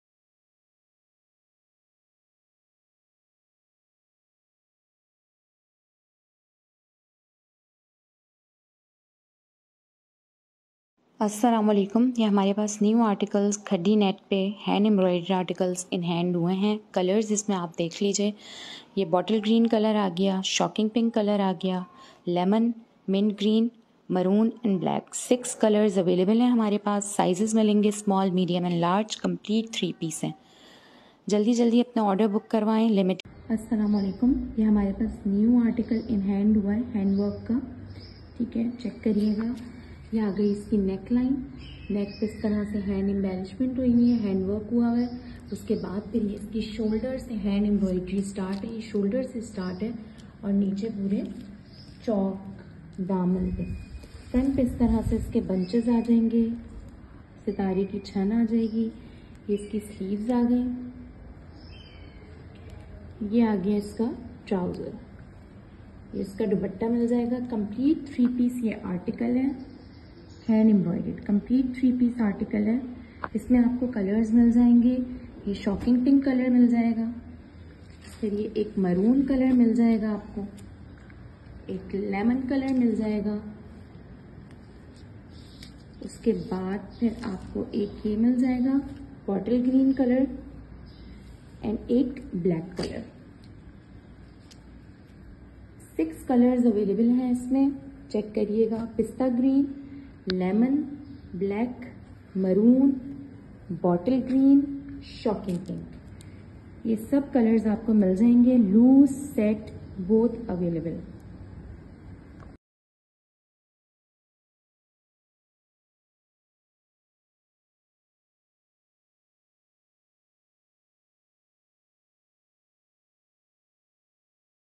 khadi net 3ps
Stitched hand embroidered shirt on khaddi new fabric paired with khaddi net dupatta with 4 side sitara  lace and Russian silk trouser..

Sizes small medium and large..

7000

Stock ready to dispatch